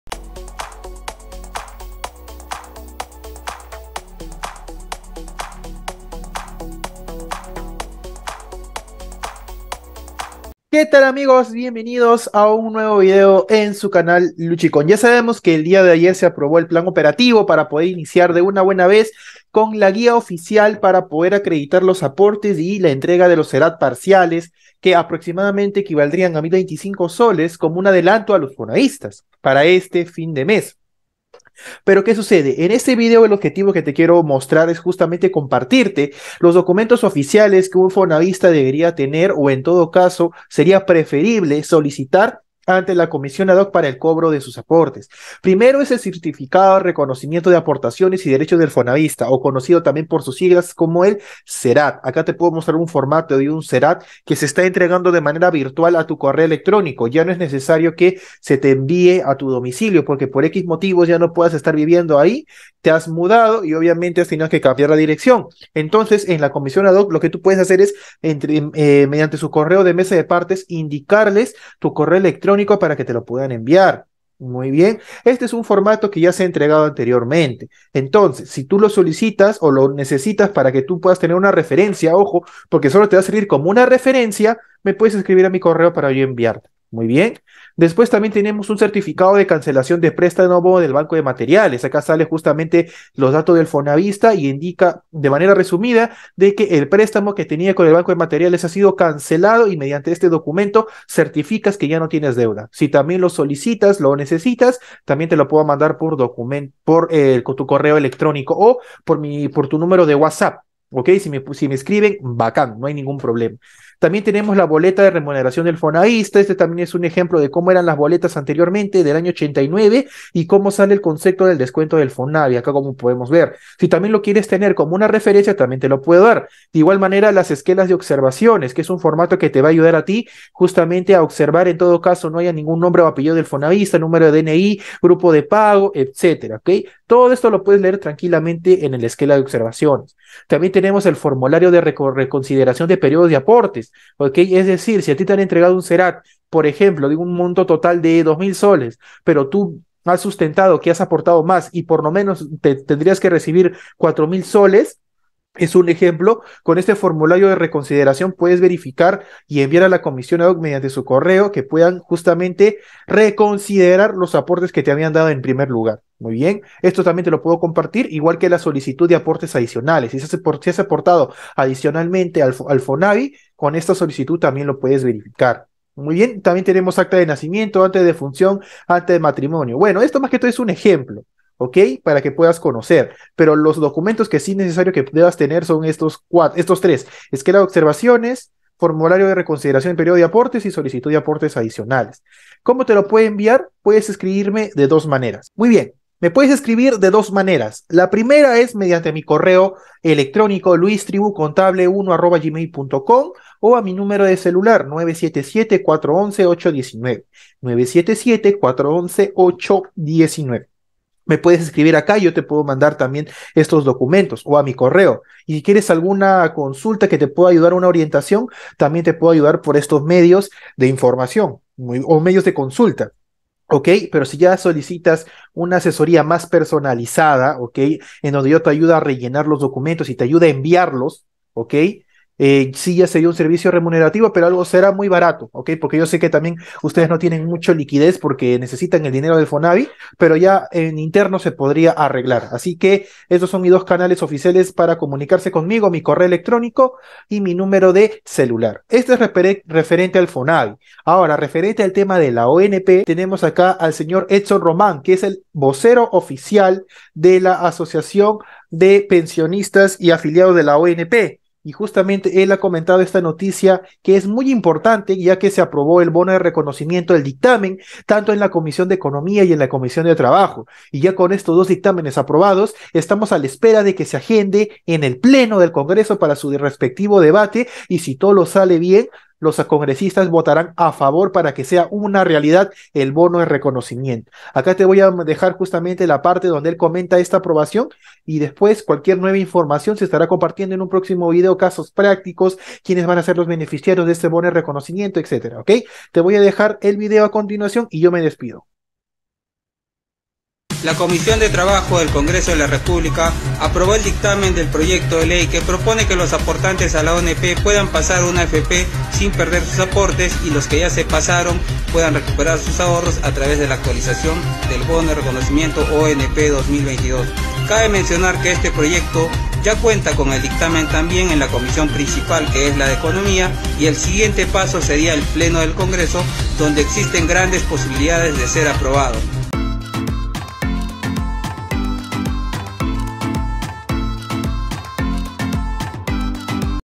*ES REALIDAD BONO ONP Y PAGO FONAVI* DOCUMENTOS OFICIALES PARA PAGO FONAVI Y DEBATE ONP EN CONGRESO
En este video te comparto las ultimas noticias sobre la ONP y el FONAVI.